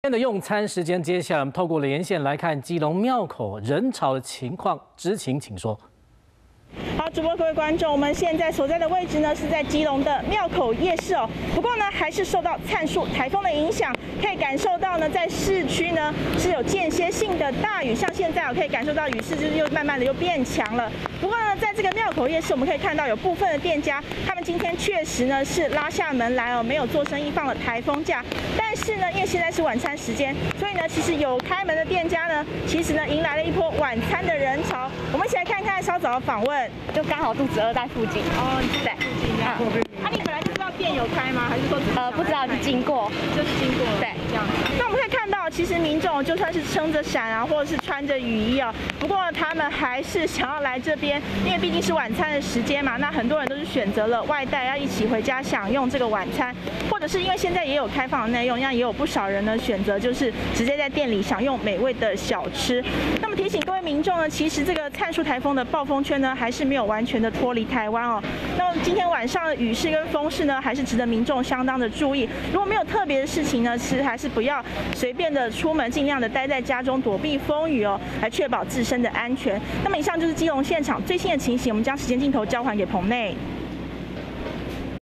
基隆風雨暫歇! 晚餐時間廟口現人潮｜華視新聞 20210912
基隆因為璨樹颱風今天停班停課，不過著名的廟口夜市還是有攤商開門做生意，晚餐時間的客人也陸續上門內用或是外帶回家享用。